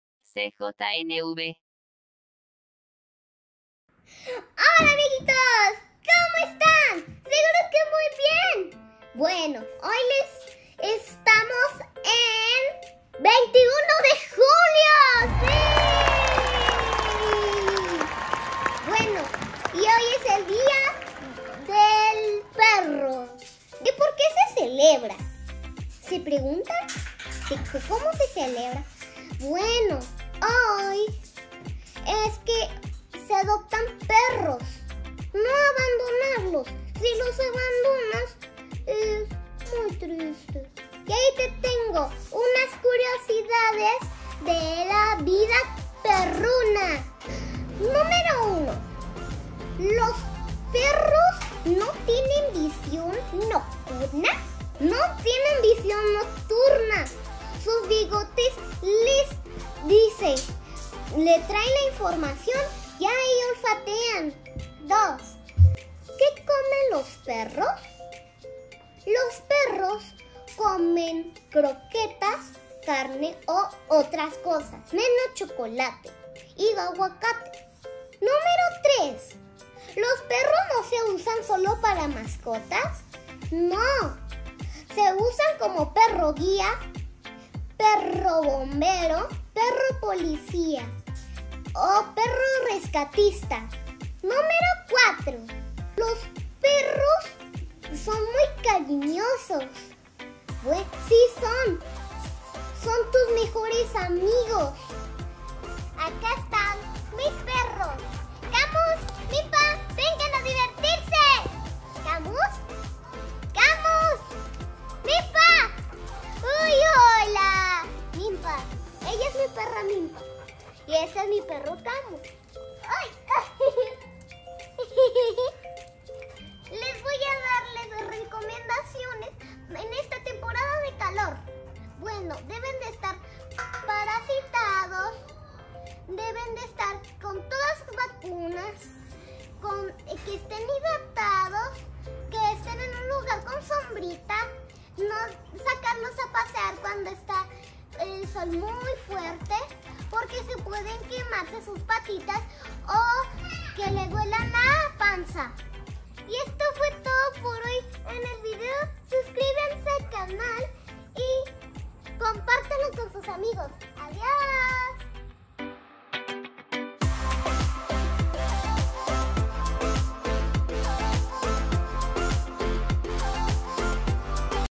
DÍA MUNDIAL DEL PERRO!! 🐾🐶
🐾¿Sabías que hoy es el día del perro?
Helena, integrante del Comité de Jóvenes y Niños Voluntarios de MUJEF A.C, te comparte datos muy interesantes de nuestros amigos peludos 🐶

Si te interesa recibir más información de este comité déjanos un mensaje y nosotros te contactamos✉